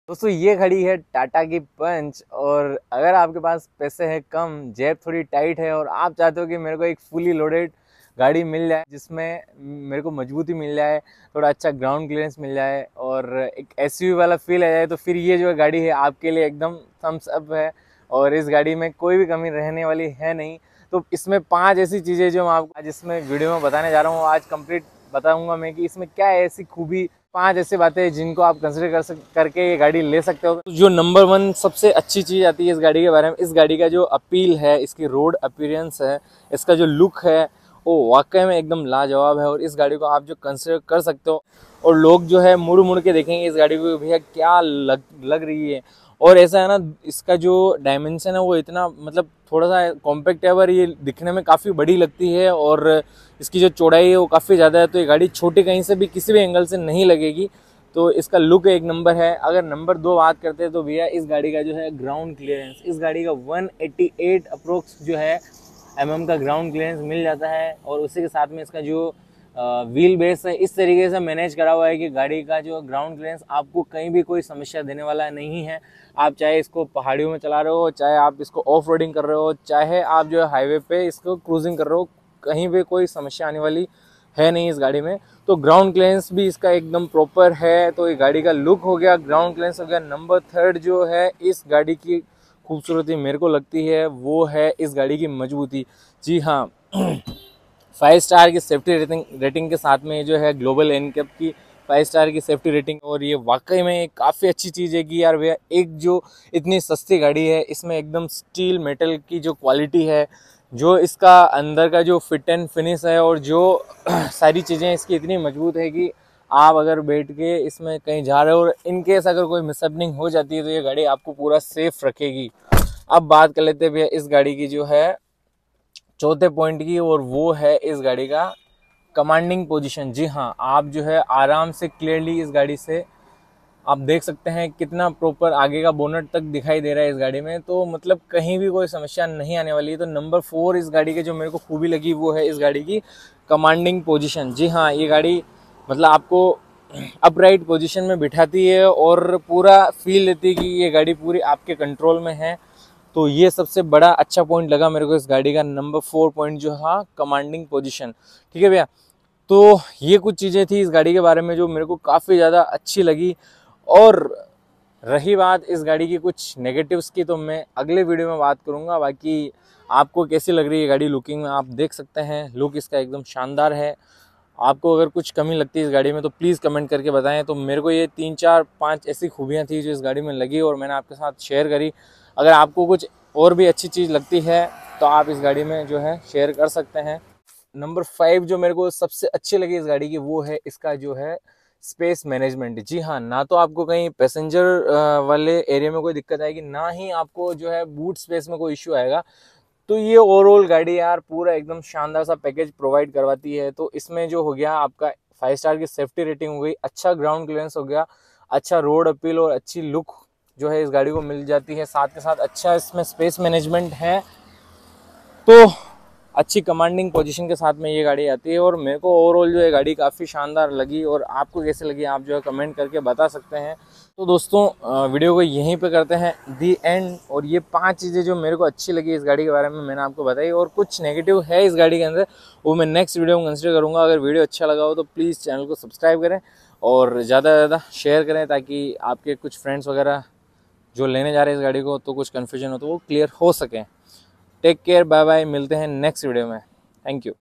TATA PUNCH Base model 2022| 5 best features| pros of punch| 6-7 लाख में इस से ज्यादा क्या मिलेगा🔥🔥
best features loded chipest car